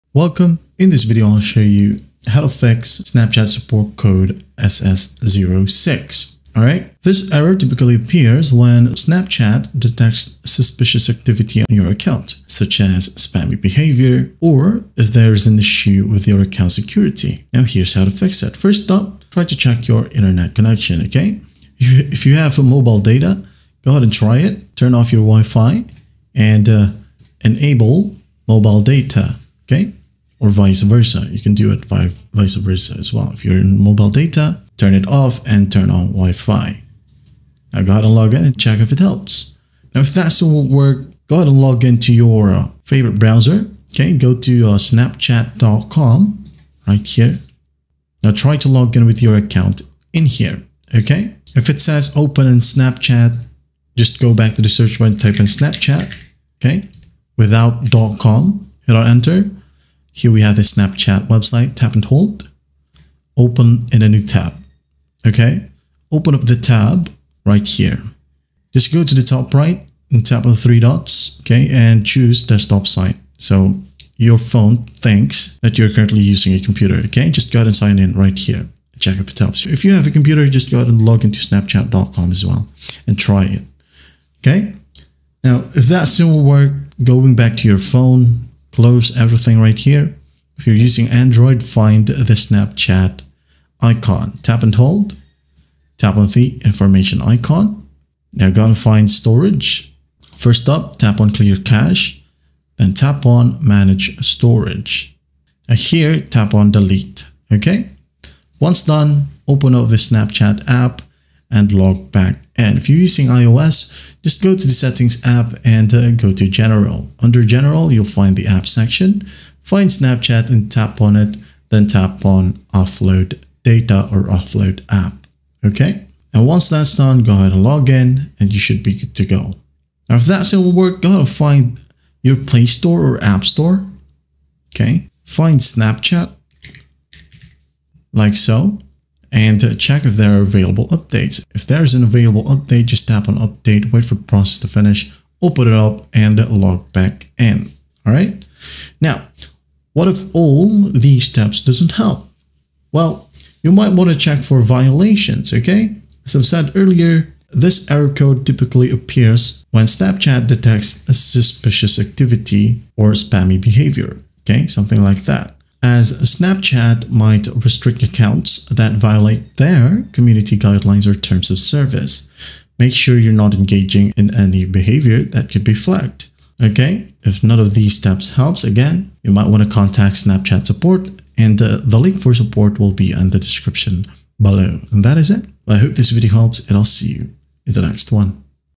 How To Fix Snapchat Support Code SS06 (Full Guide 2026)
How To Fix Snapchat Support Code SS06

In this video, I will show you how to fix Snapchat Support Code SS06. This error usually appears when your account has been temporarily locked, restricted, or flagged for unusual activity. I’ll guide you through several methods to solve the problem, including verifying your account, resetting your password, using the official Snapchat Unlock page, and avoiding third-party apps that may cause the error. These solutions work on both iPhone and Android devices. By the end, you’ll know exactly how to fix the SS06 support code issue and regain access to your Snapchat account.

Chapters:
00:00 Introduction
00:10 How To Fix Snapchat Support Code SS06